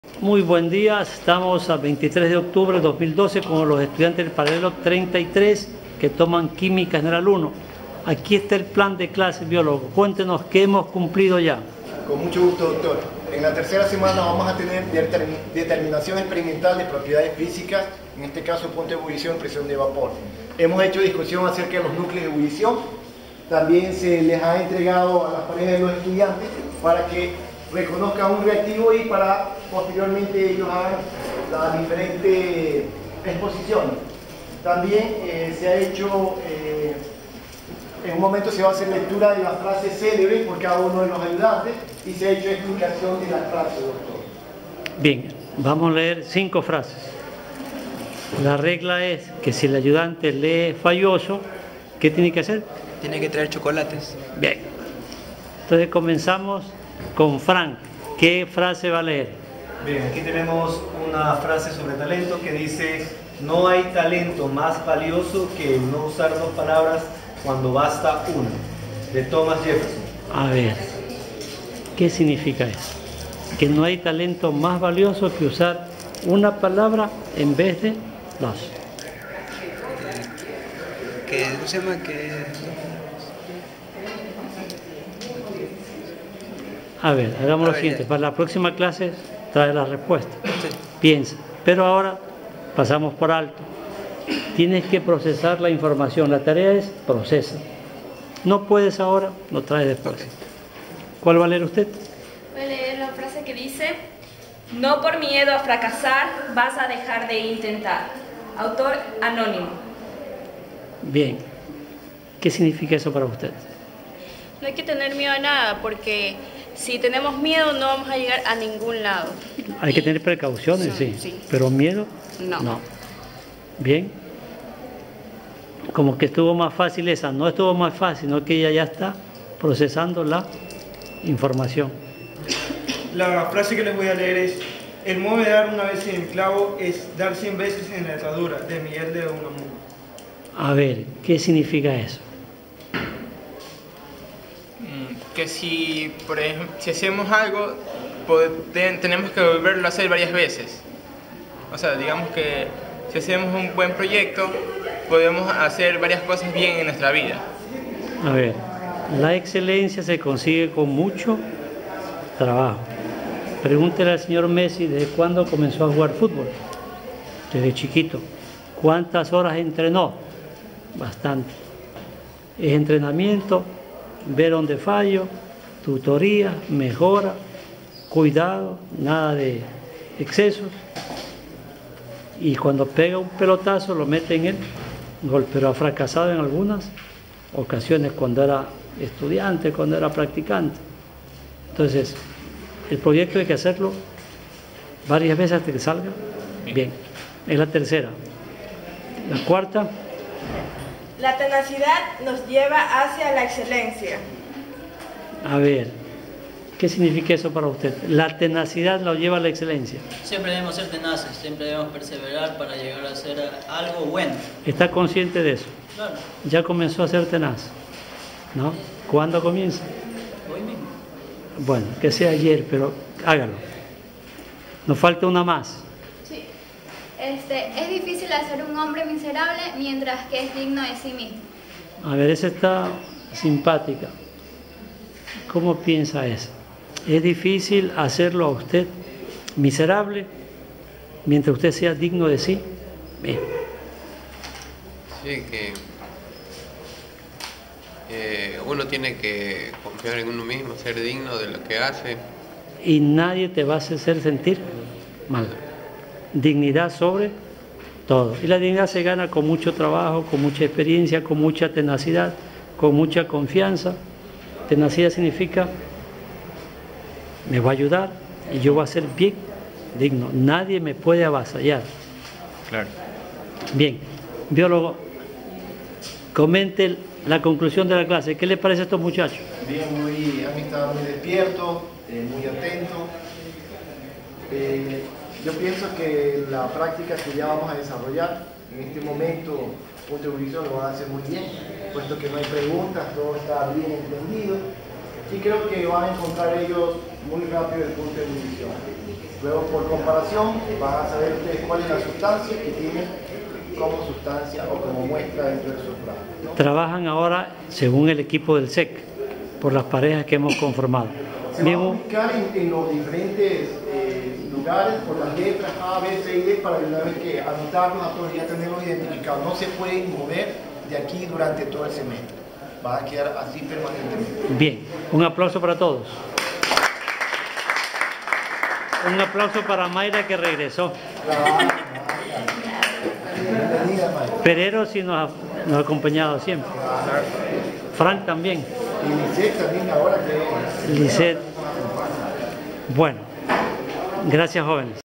#2/2 Química General I ESPOL p009 p033 3er día, ESPOL, ICQA, CSECT, 2012.10.23.
4to día de clases en el ICQA (ESPOL), ver resumen de clase.
Presión de vapor, puntos de ebullición.
Frases célebres.